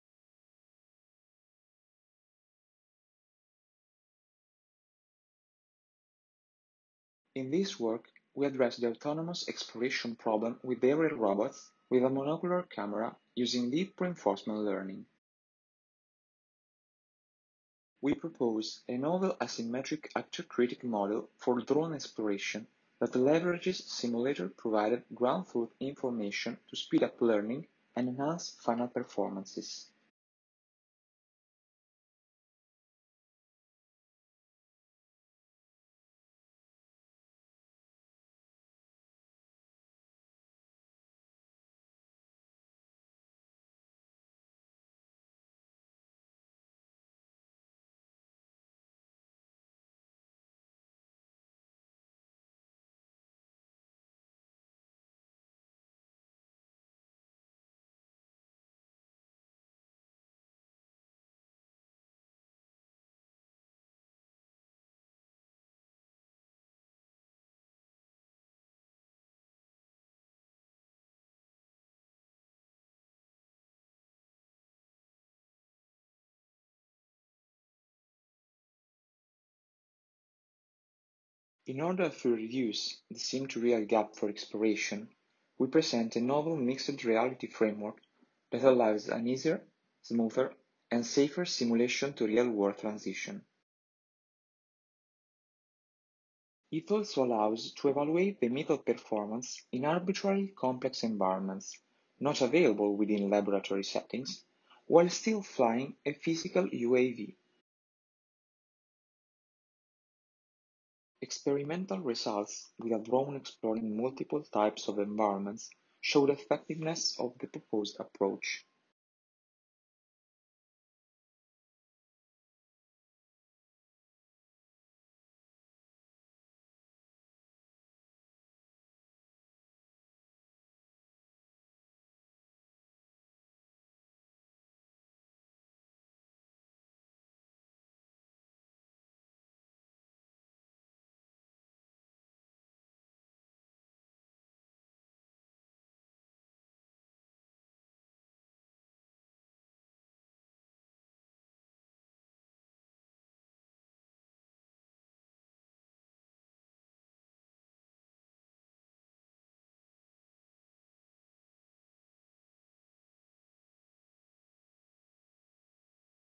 Autonomous Single Image Drone Exploration with Deep Reinforcement Learning and Mixed Reality
Autonomous exploration is a longstanding goal of the robotics community. The stringent requirements on cost, weight, maneuverability, and power consumption do not allow exploration approaches to easily be employed or adapted to different types of environments. End-to-End Deep Reinforcement Learning techniques based on Convolutional Networks, which grant constant-time computation, predefined memory usage, and deliver high visual perception capabilities, represent a very promising alternative to current state of the art solutions relying on metric environment reconstruction. In this work, we address the autonomous exploration problem with aerial robots with a monocular camera and propose a novel asymmetric actor-critic model for drone exploration that efficiently leverages ground truth information provided by the simulator environment to speed up learning and enhance final exploration performances. Furthermore to reduce the sim-to-real gap, we present a novel mixed reality framework that allows an easier, smoother, and safer simulation to real-world transition. Both aspects allows us to further exploit the great potential of simulation engines and reduce the risk associated with directly deploying algorithms on a physical platform with no intermediate steps. This is well-known to create several safety concerns and be dangerous when deploying aerial vehicles. Experimental results with a drone exploring multiple environments show the effectiveness of the proposed approach.